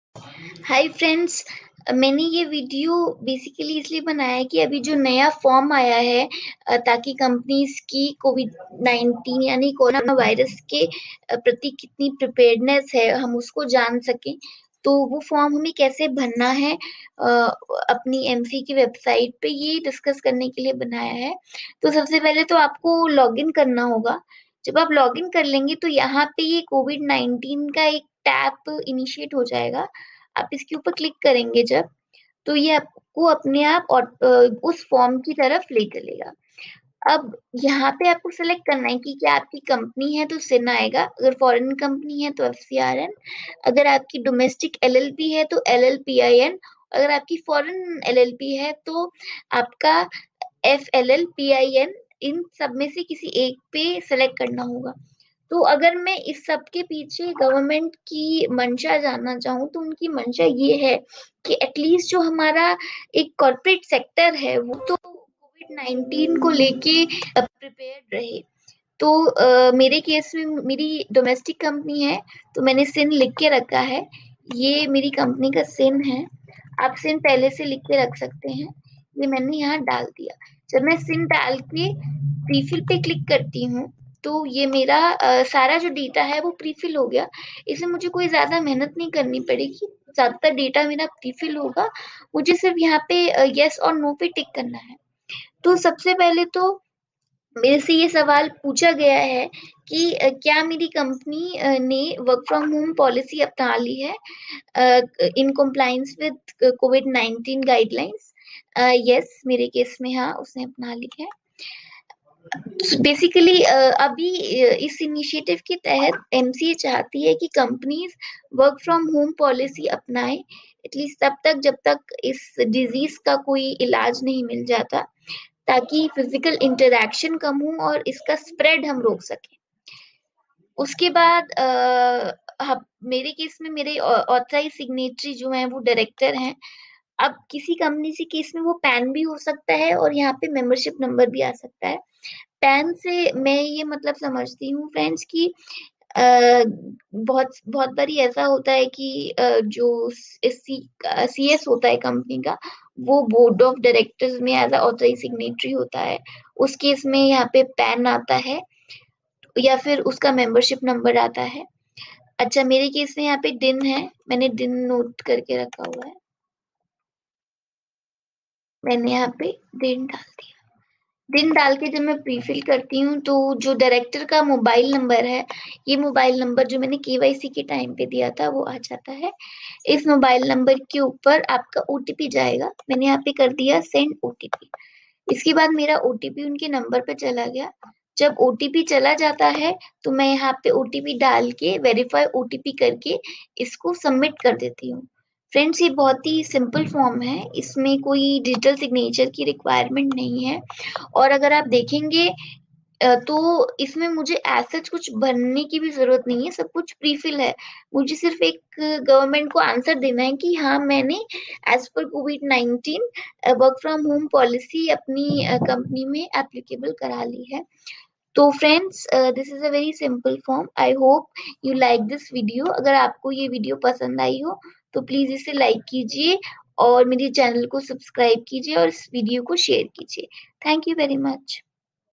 Step by step Guidelines to fill MCA E-form COVID-19
Ministry of Corporate Affairs vide a notification dated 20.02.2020, to all the Corporates including Companies and LLPs both are expected and strongly advised to put in place an immediate plan to implement “Work from Home” Policy in their headquarters and field offices.